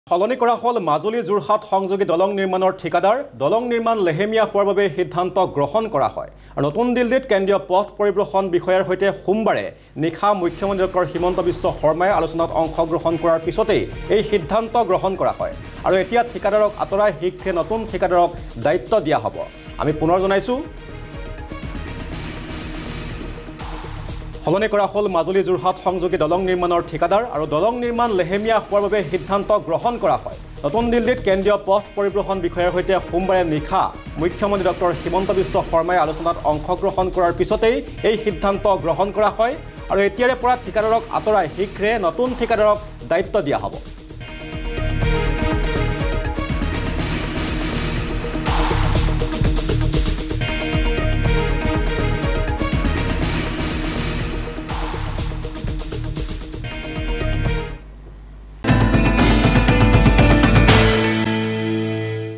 মাজুলী-যোৰহাট দলং নিৰ্মাণৰ ঠিকাদাৰ সলনি
📌 কেন্দ্ৰীয় পথ পৰিবহণ বিষয়াৰে মুখ্যমন্ত্ৰীৰ বৈঠকৰ পাছতে এই সিদ্ধান্ত
📌লেহেমীয়া কাৰ্যৰ বাবে ঠিকাদাৰ সলনিৰ সিদ্ধান্ত, শীঘ্ৰেই নতুন ঠিকাদাৰক দায়িত্ব প্ৰদান কৰা হ'ব